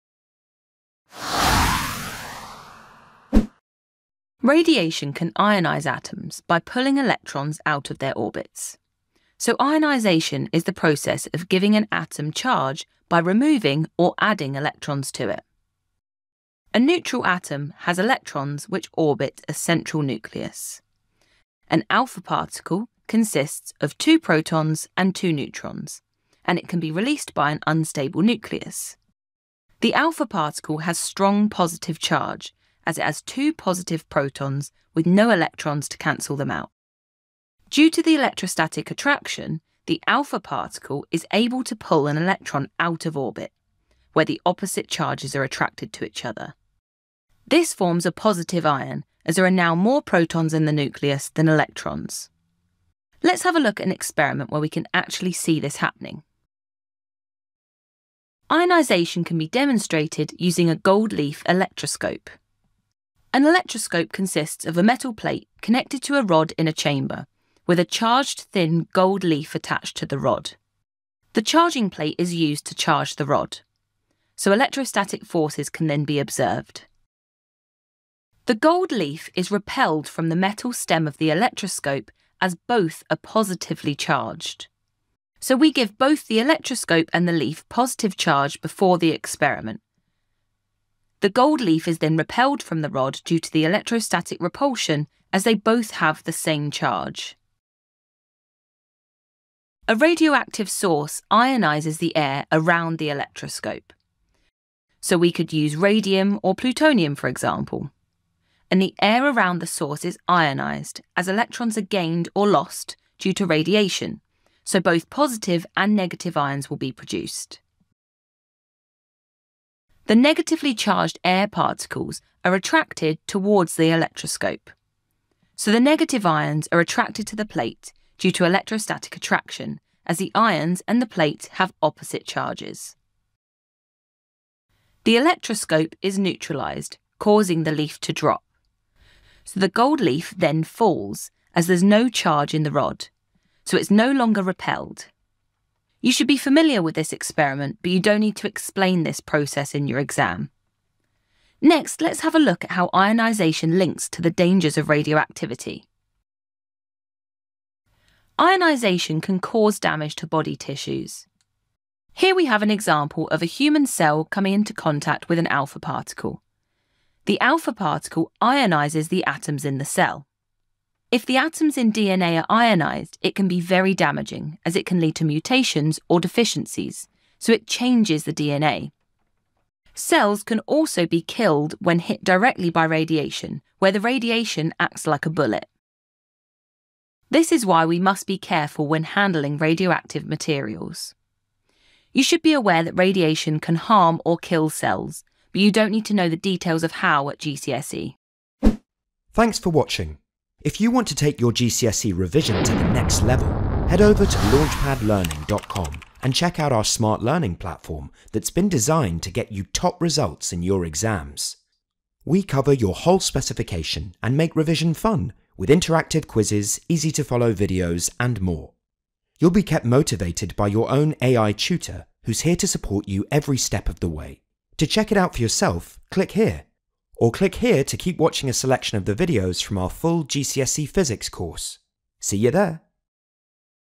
Ionisation - GCSE Physics
🦊 In this GCSE Physics video, we explain that ionization occurs when radiation, such as alpha particles, removes electrons from atoms, creating charged ions. This process can be demonstrated using a gold leaf electroscope, where ionization causes the leaf to drop as the electroscope becomes neutralized. It also discusses the potential dangers of ionization, as it can damage DNA and cells, highlighting the importance of handling radioactive materials carefully.